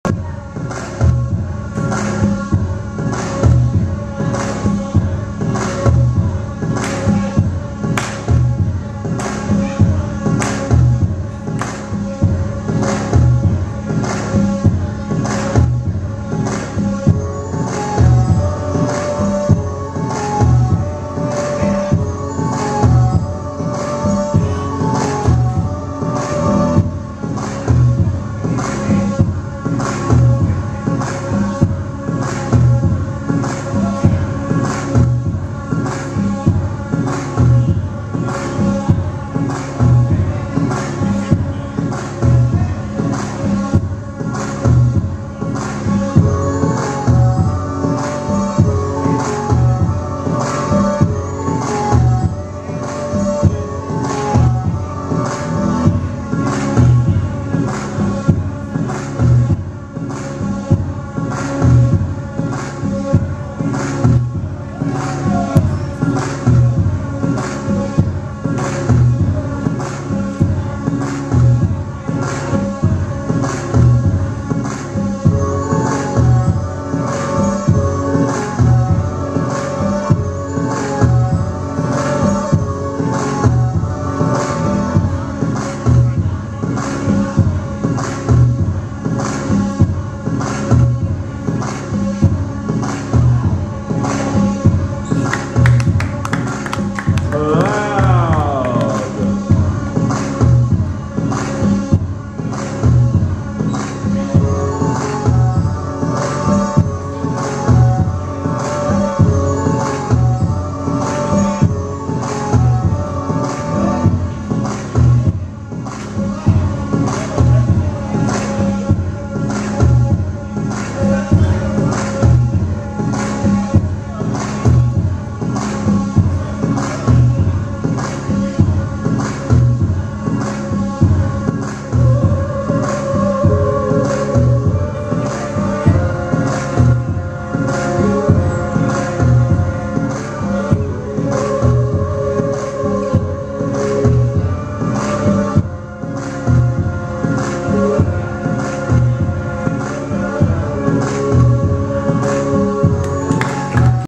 J Fung v Cloud
YA's All Styles Battles on Tuesday, January 24, 2012 at Gethsemane Church in Houston, Texas